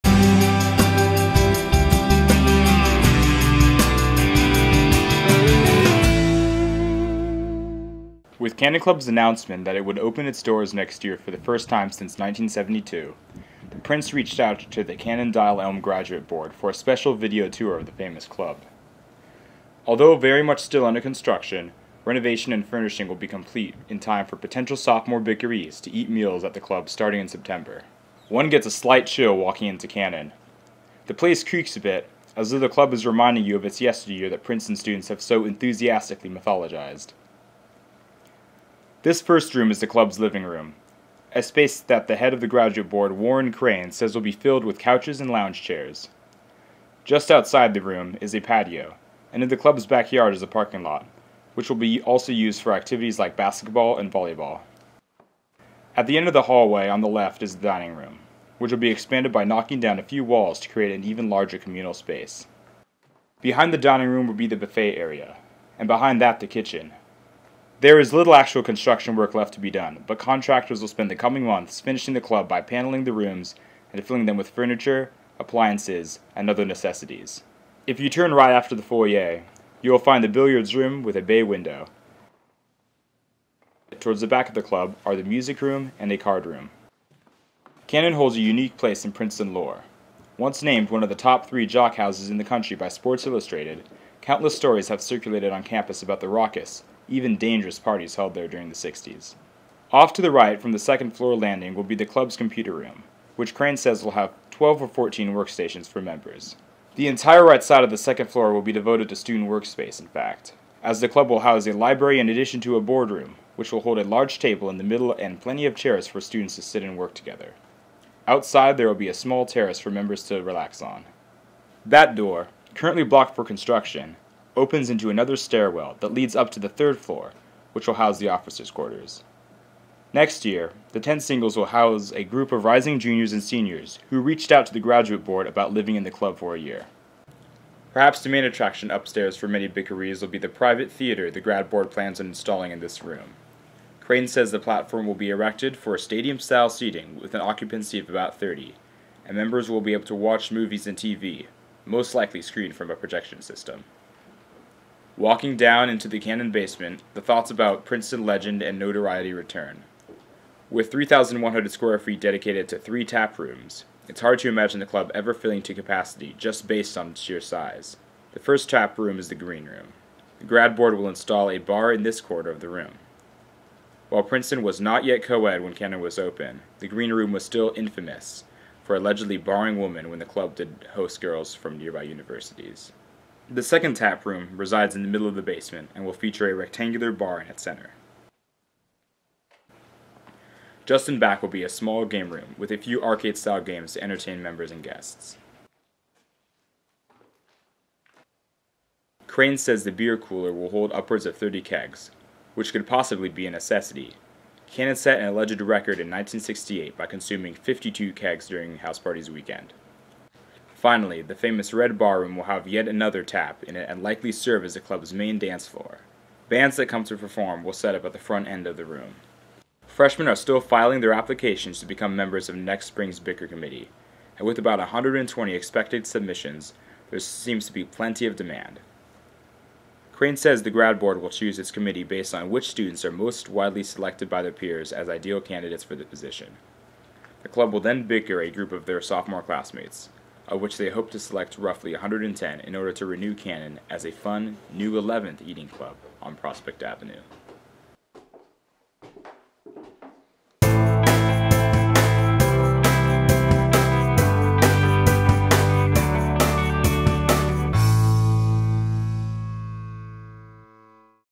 Cannon Video Tour
The Prince takes you inside Cannon for an exclusive video tour of the infamous club, which will reopen next year for the first time since 1972.